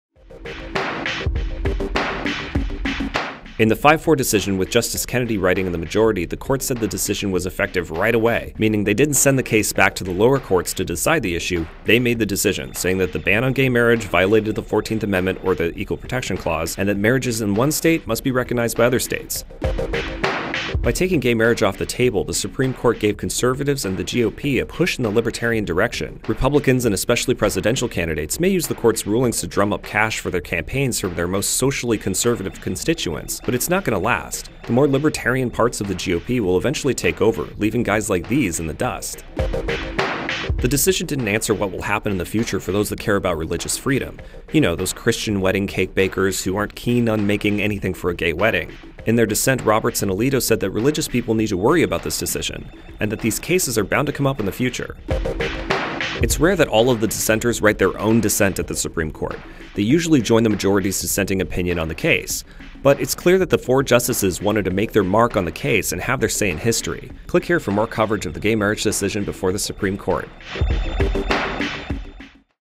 4 Takeaways from SCOTUS Gay Marriage Ruling
The Supreme Court of the United States ruled in a 5-4 ruling in favor of gay marriage. While the decision will be debated for years to come, watch some of our takeaways from the ruling.

Written and produced by Paul Detrick